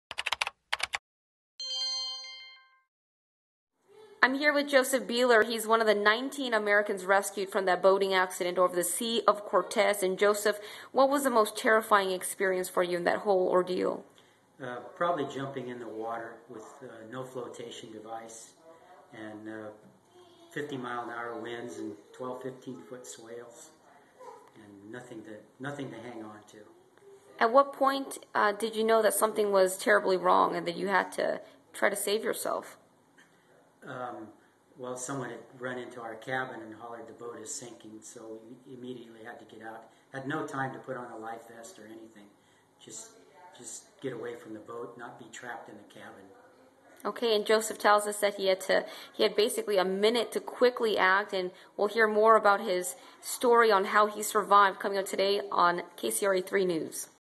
From The Field: Ripon Man Describes How He Survived Boating Accident
Joseph Beeler, 59, is one of the lucky 19 who was rescued after a boat capsized during a fishing trip. He's not at his Ripon home.